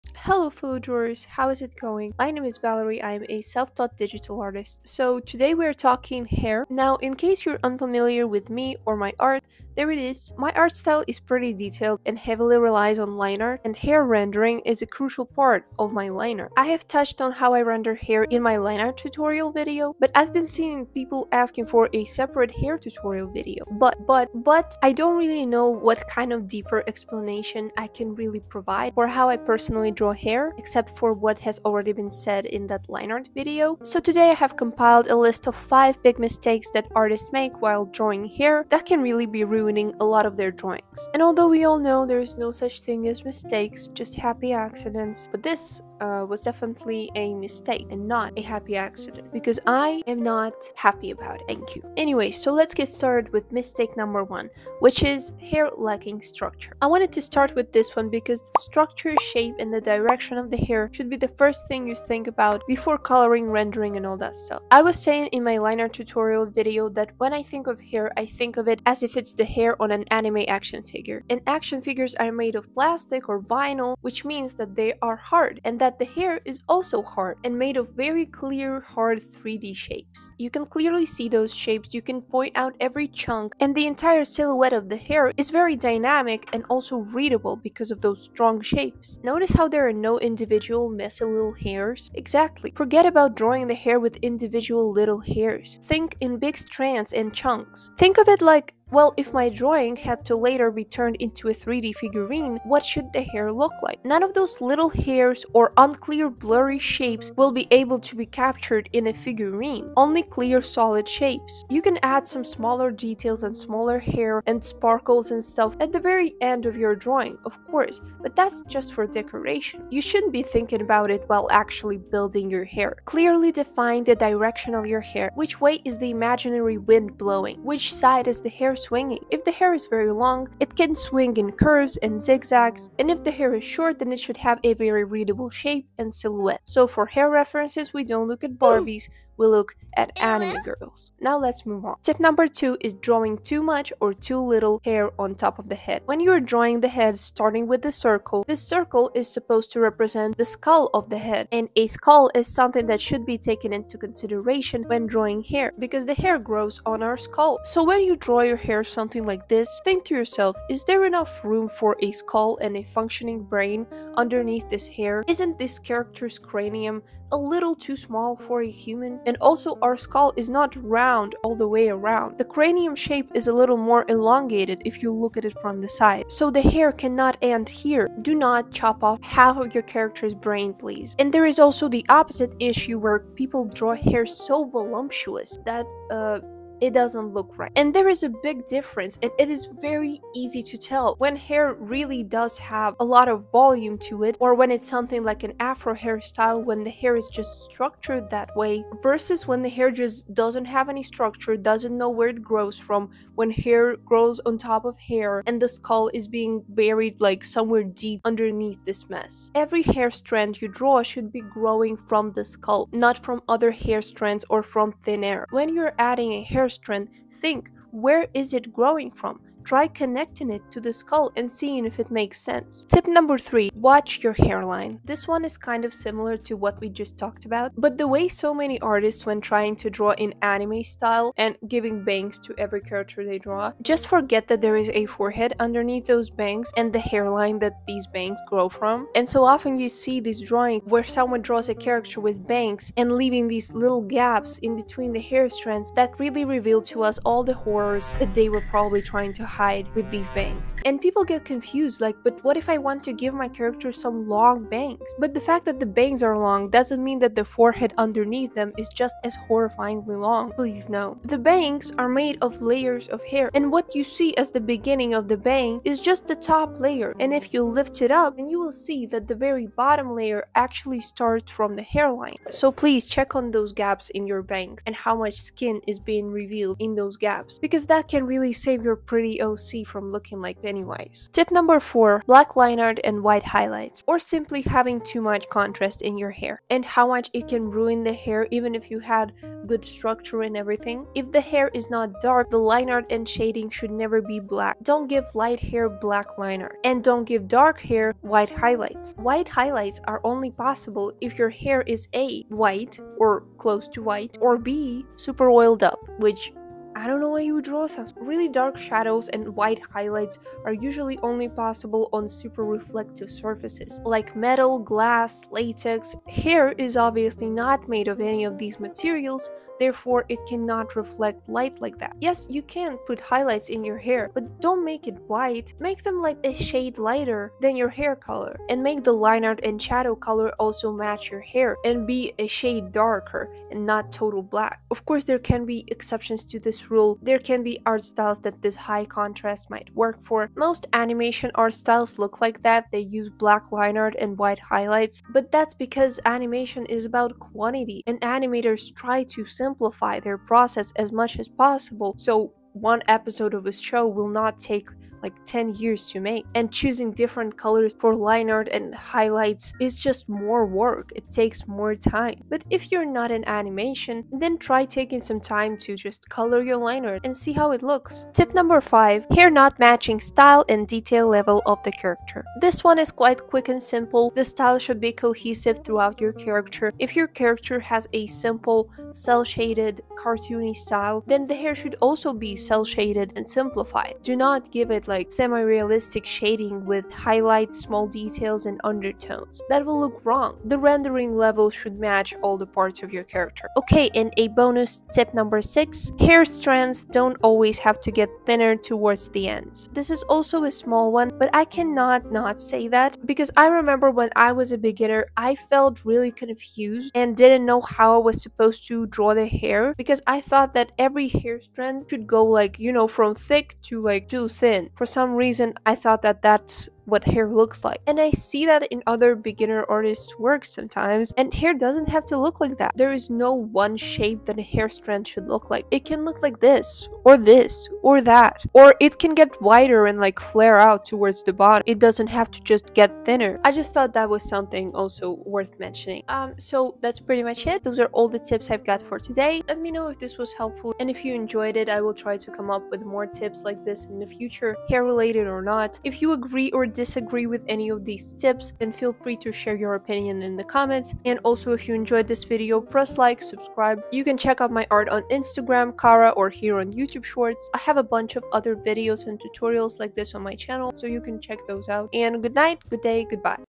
5 BIG MISTAKES artists make when drawing HAIR
in this video i'll tell you about 5 big mistakes beginner artists make when drawing hair , that can be ruining their drawings, and how to fix them

0:00 intro
0:51 mistake 1
2:21 mistake 2
3:42 tip? 3
4:41 mistake 4
6:08 mistake 5
6:33 bonus tip? 6